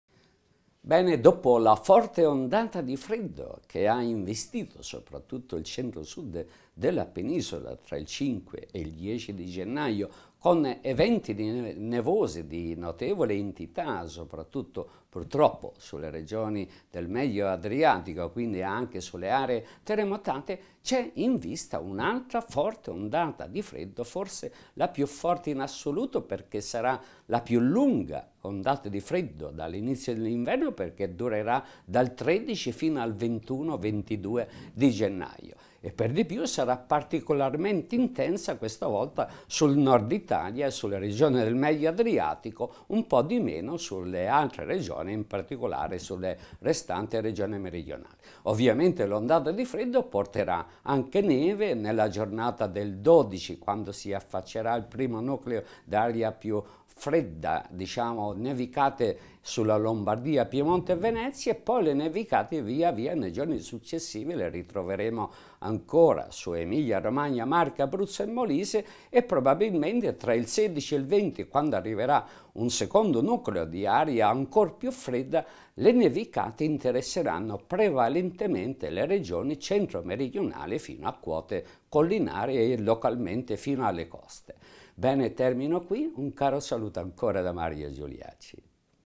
Nuova intensa ondata di gelo in arrivo
Nei prossimi giorni di nuovo freddo intenso e nevicate fino a bassa quota in molte zone d'Italia, comprese le regioni settentrionali.